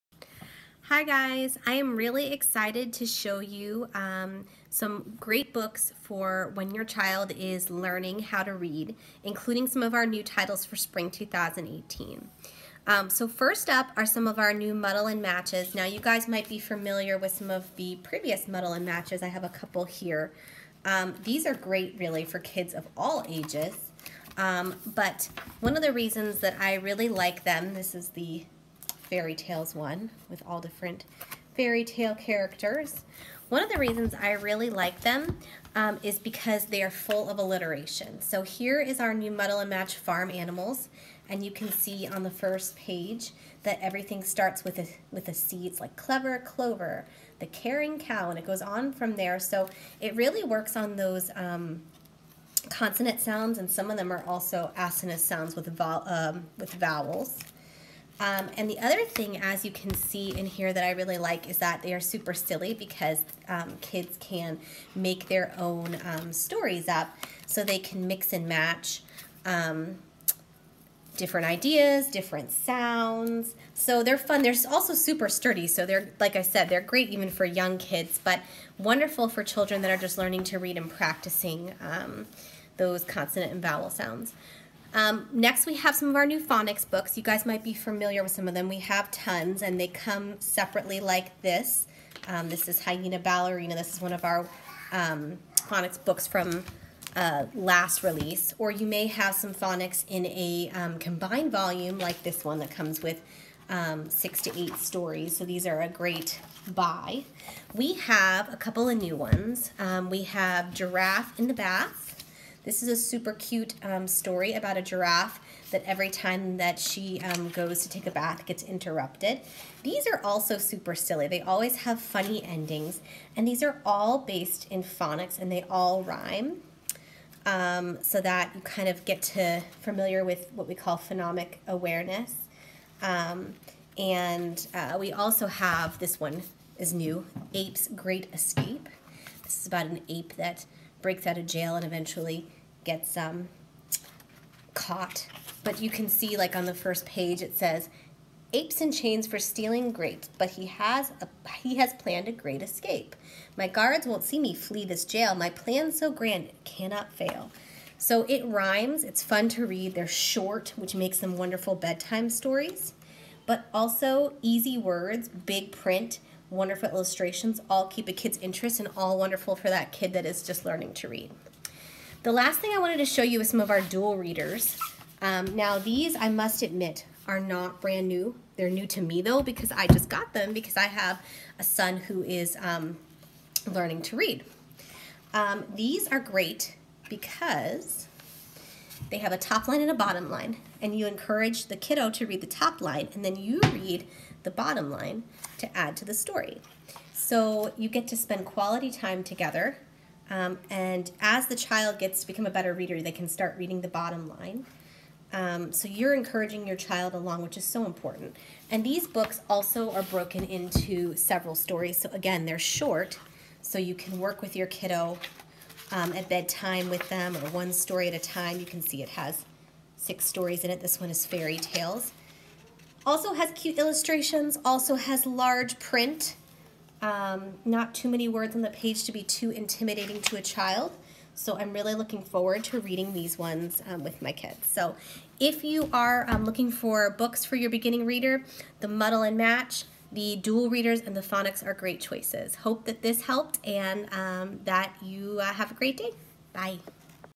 Usborne Books & More Books for Beginning Readers- includes Spring 2018 Titles!
Usborne Books & More Books for Beginning Readers! Phonics books, Muddle and Match and Dual Readers!